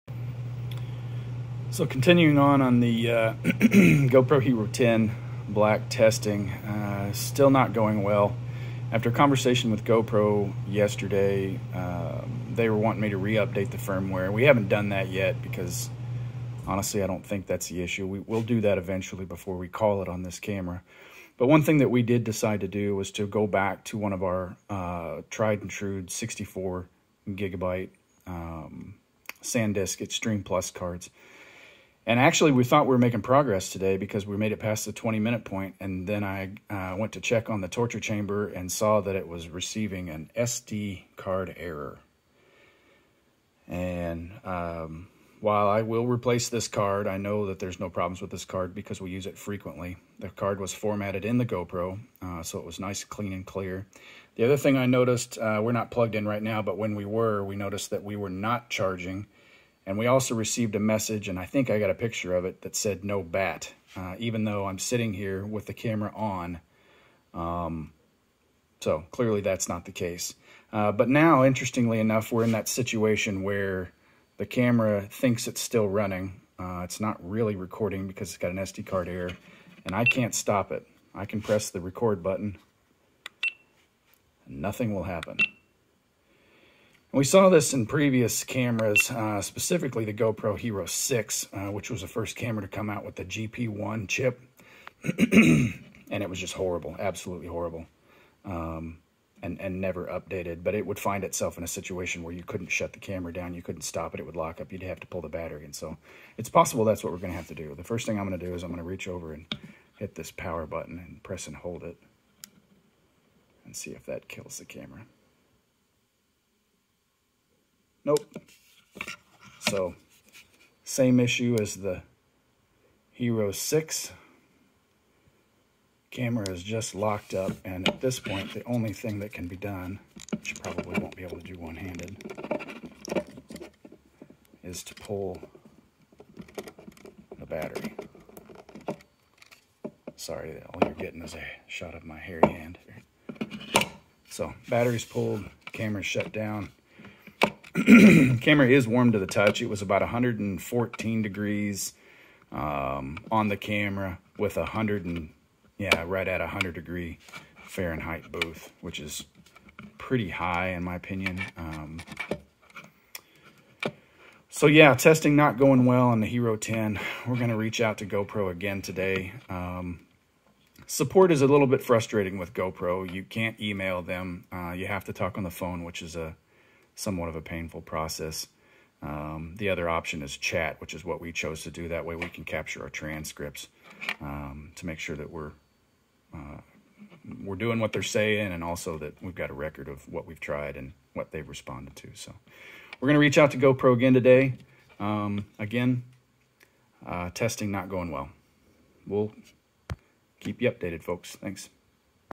GoPro HERO 10 SD Err
As we continue to put the HERO 10 through the testing phase, we are finding more bugs and problems.  in this example, we show an SD Err message (on a good card) as well as a camera lock up.  These issues are similar to the problems on the HERO 6 when GoPro came out with the GP1 chip.

Proudly Made in the USA since 2013